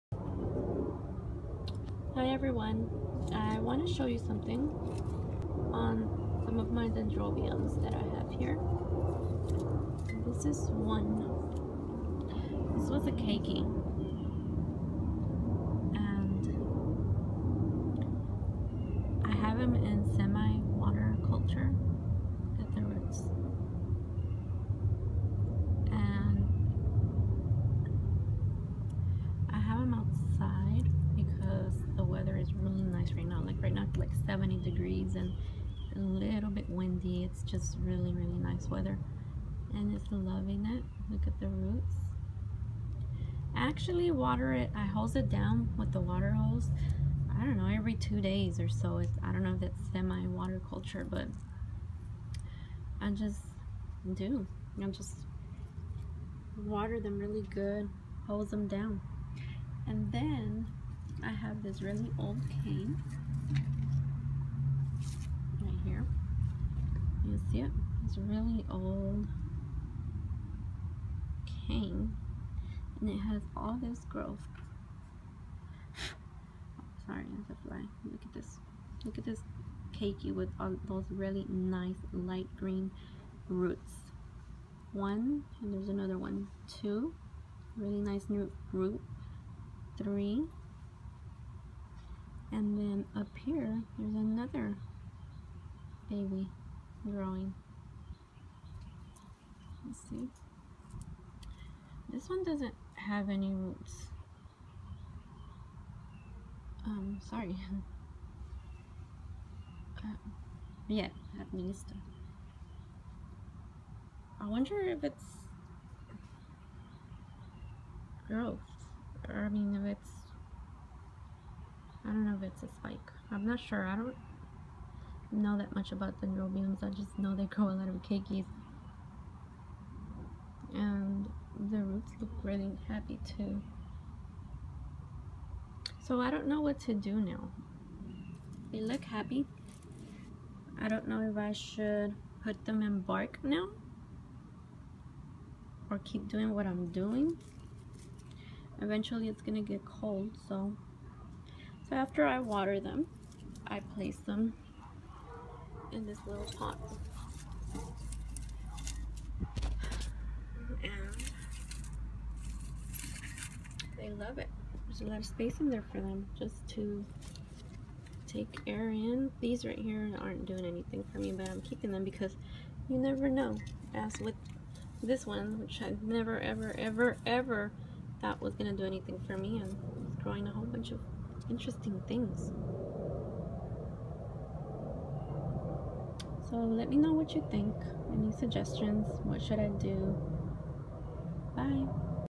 what should I do ?
with these dendrobiums ? bark ? LEave as is ?